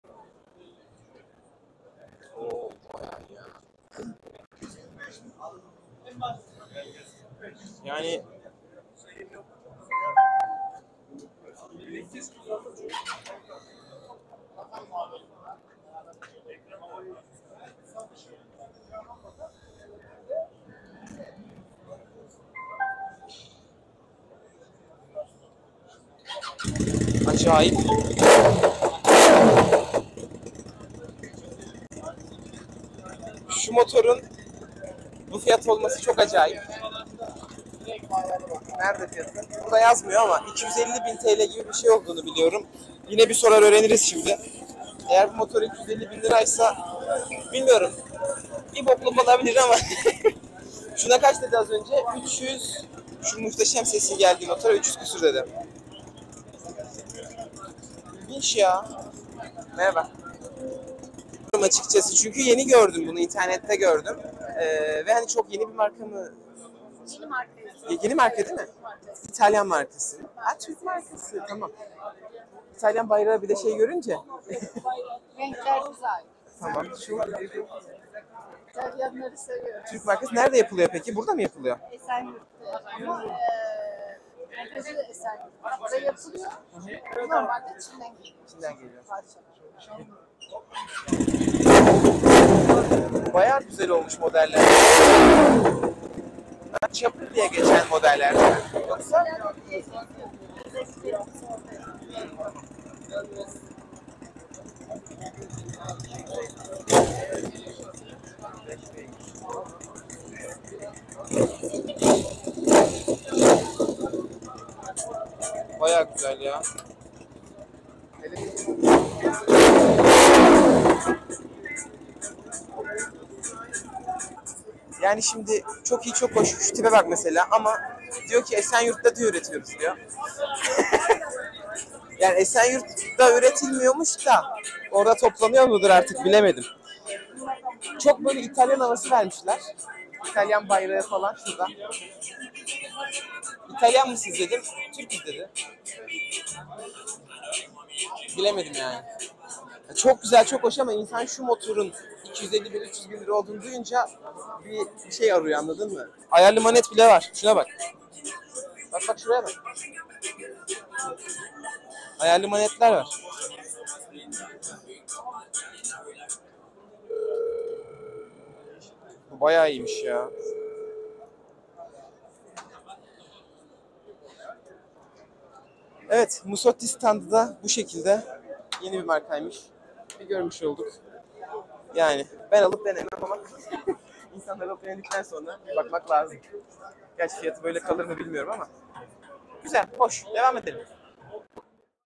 Musatti Yeni Bir Marka! İtalyan mı? Türk mü? İstanbul Motosiklet Fuarı 2024
Motobike İstanbul 2024 fuarında Musatti ile karşınızdayız. Musatti 2020 yılı itibarıyla motosiklet sektörüne adım atmış bir markadır.
Milanio-250 ve KAI-ZEN modellerini inceledik. Keyifli seyirler dileriz.